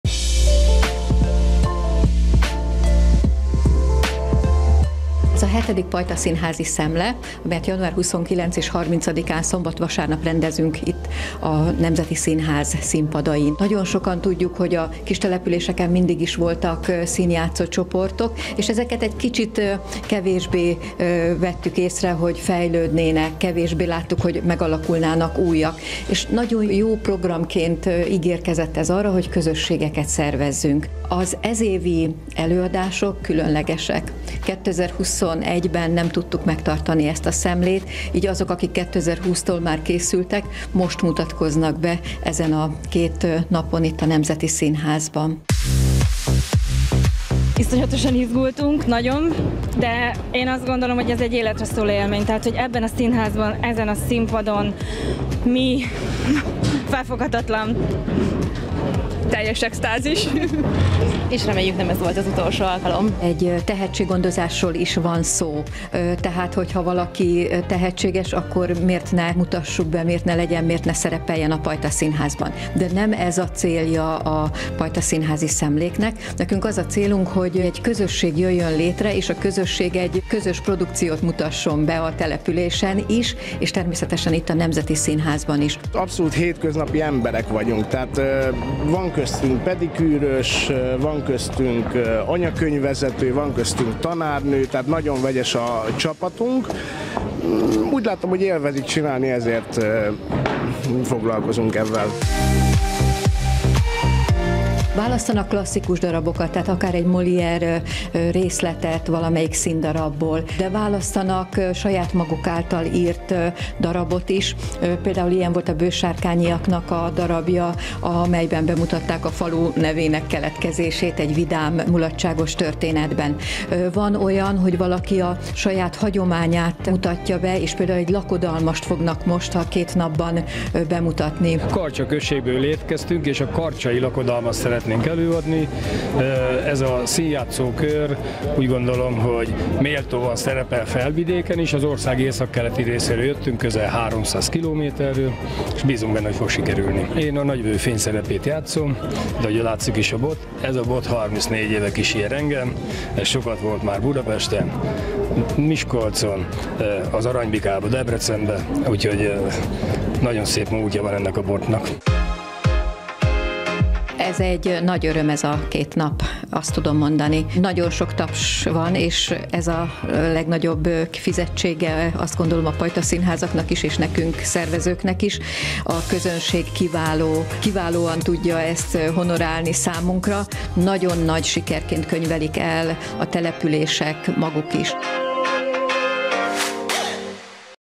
Pajtaszínházi Szemle a Nemzeti Színházban
Pajtaszínházi Szemle a Nemzeti Színházban – immár hetedszer! A Nemzeti Művelődési Intézet és a Magyar Teátrumi Társaság közös kezdeményezése a falusi amatőr színjátszás örömünnepe: 2 napon keresztül professzionális színpadi környezetben mutatkozhatnak be a vidéki amatőr társulatok egymásnak - és az érdeklődőknek.  Nézzünk szét a nyitónapon!